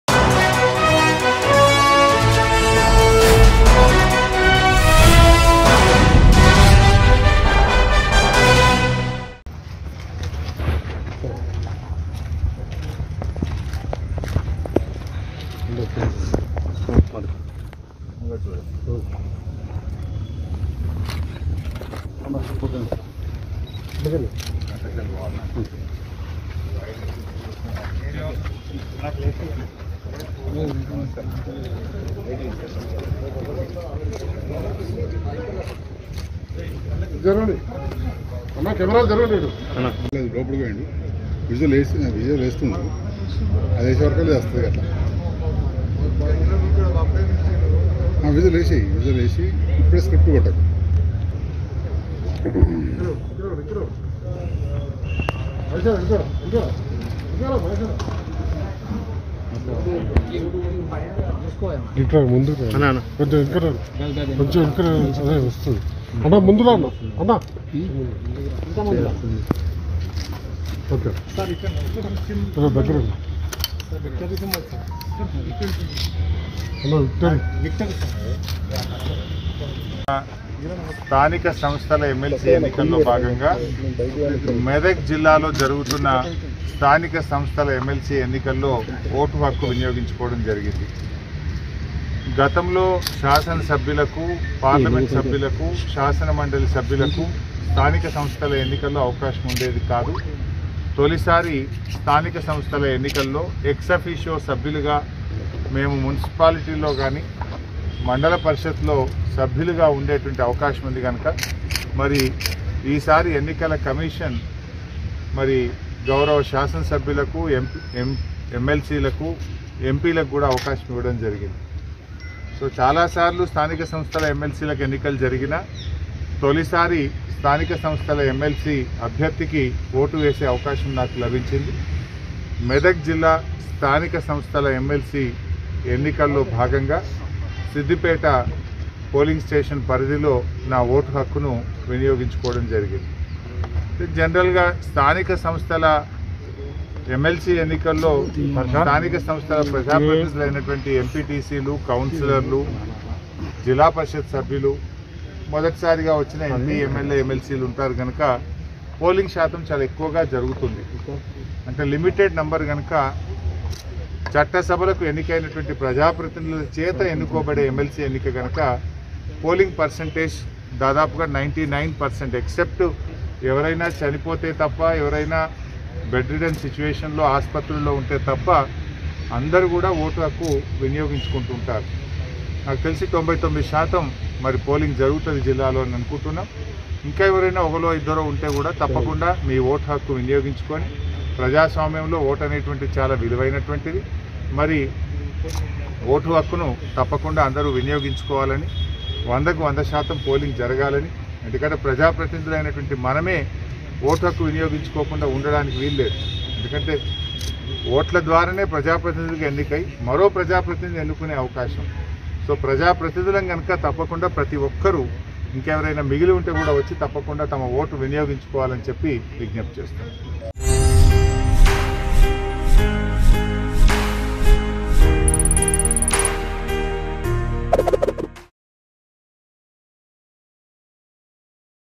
Minister Harish Rao Casts His Vote At Siddipet For MLC Elections | MLC Election Updates | Mango News
Minister Harish Rao Casts His Vote At Siddipet For MLC Elections.